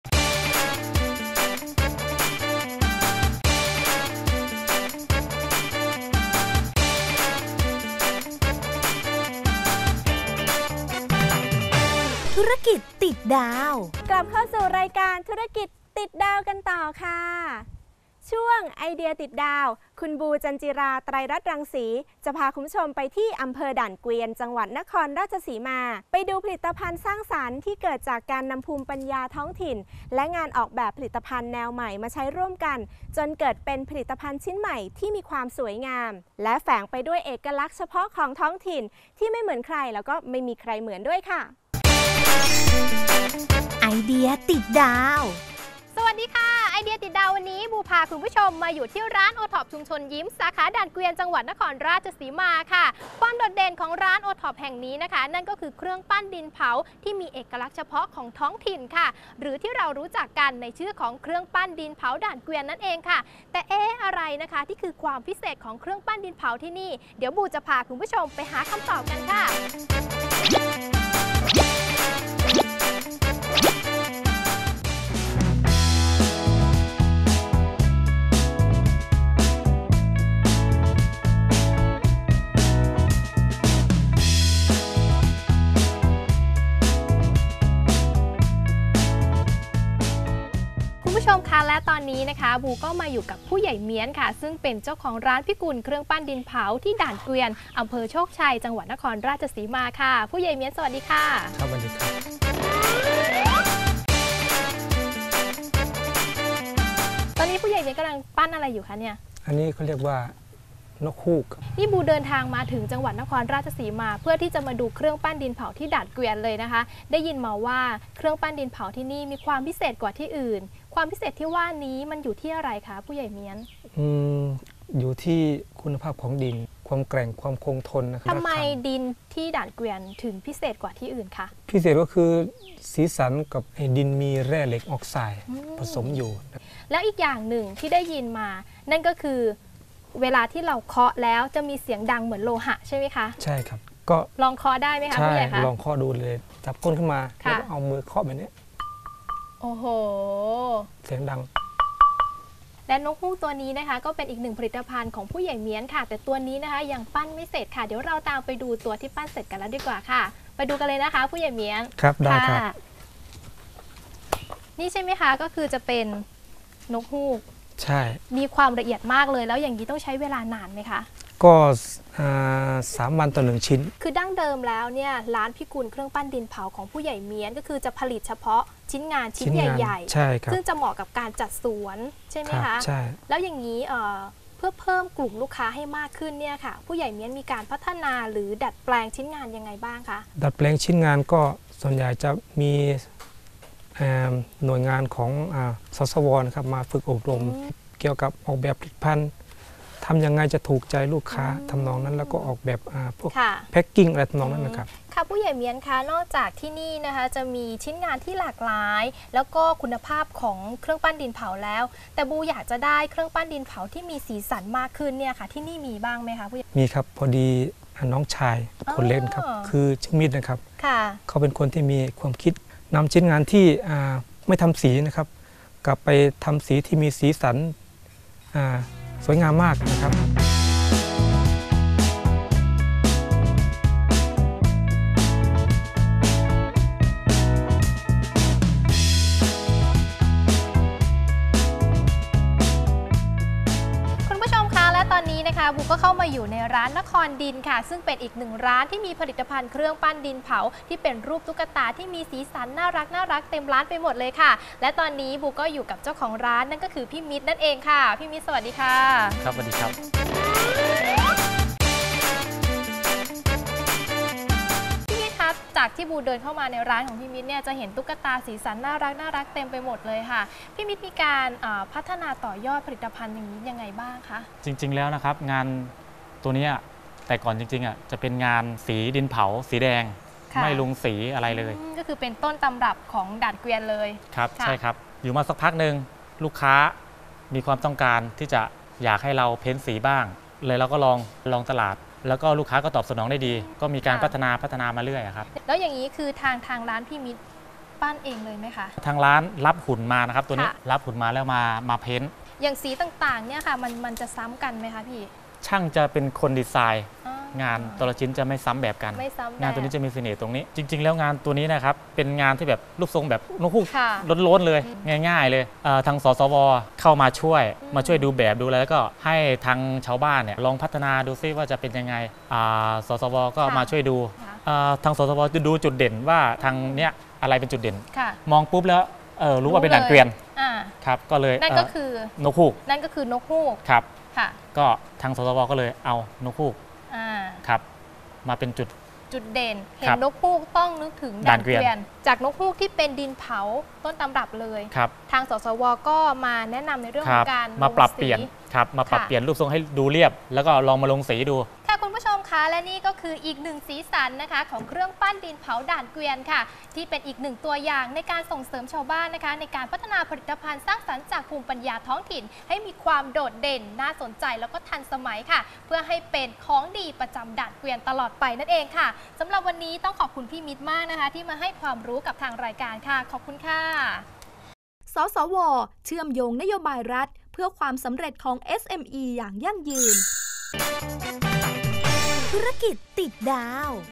สีสันเครื่องปั้นด่านเกวียน
พบกับไอเดียการพัฒนาต่อยอดผลิตภัณฑ์เครื่องปั้นดินเผาด่านเกวียน ให้มีสีสันและความโดดเด่นมากยิ่งขึ้น